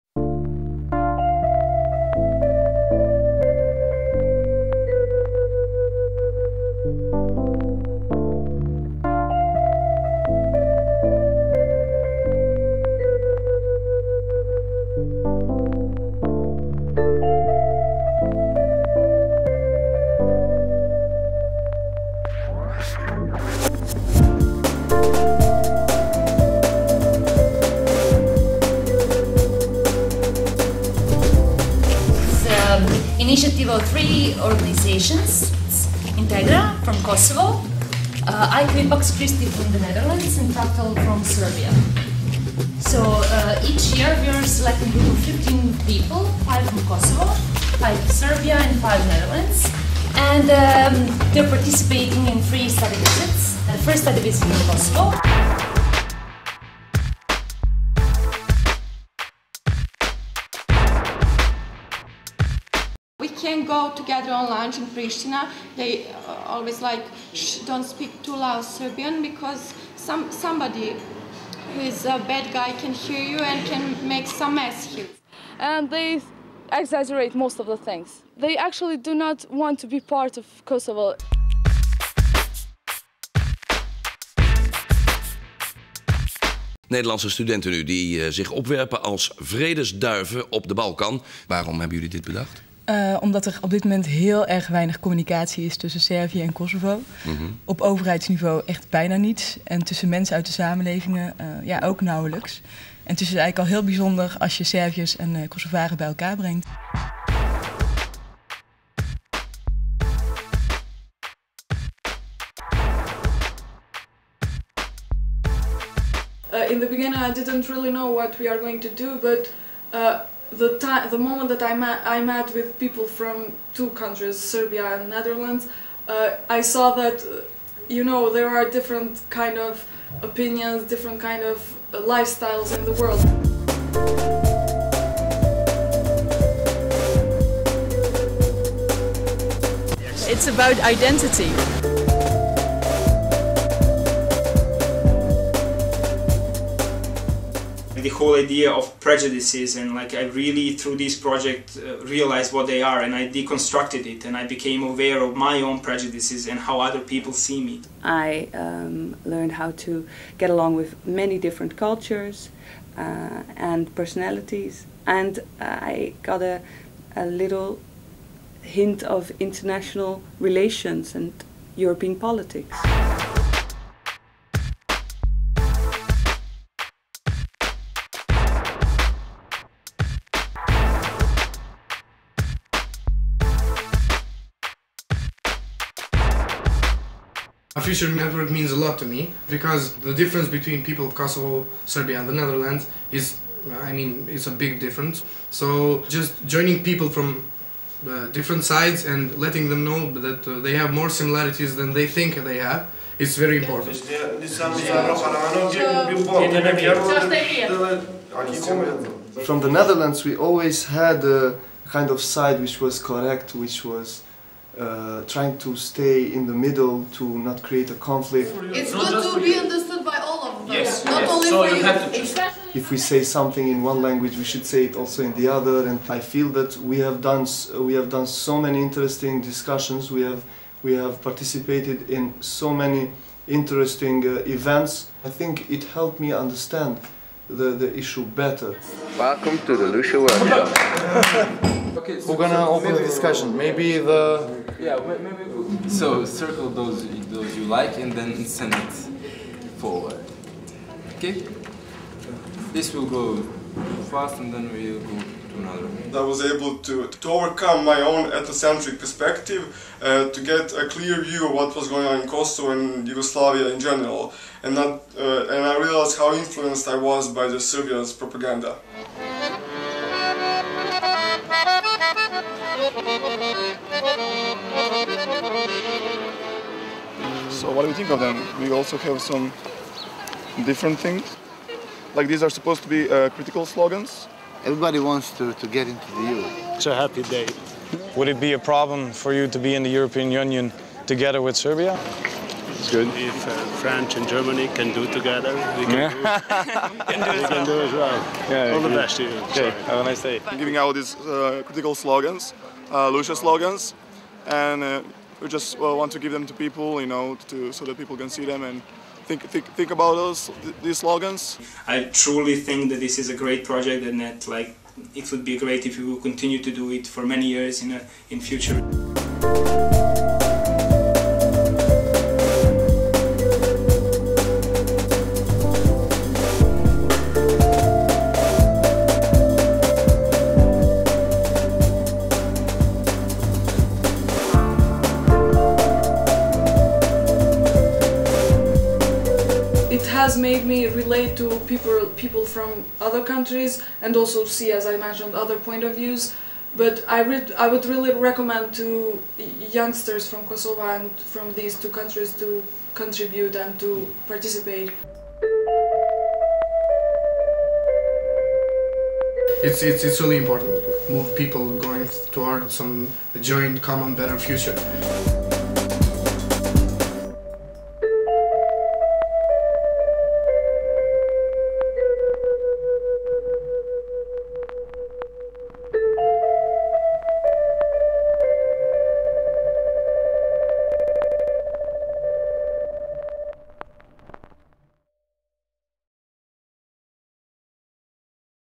Our Future Network: expectation & questions from OFN alumni and medley
Our Future Network (OFN) is an exchange program in which students from Kosovo, Serbia and The Netherlands are linked and discuss past, present  and future.
On Thursday 16th of December IKV Pax Christi, Our Future Network and Platform Spartak organized a BalkanBorrel in which European Union Special Representative Mr. Pieter Feith spoke about his work and the EU's vision for this upcoming dialogue. Also present was Marko Prelec (Balkans Project director, International Crisis Group) who gave a reaction on the lecture of Feith.
During the evening a video was shown of OFN-alumni and their expectations on the upcoming EU-mediated  dialogue. During the Q&A  clips were shown with questions to Pieter Feith from OFN-alumni. During the network café a medley was shown with old video material of OFN.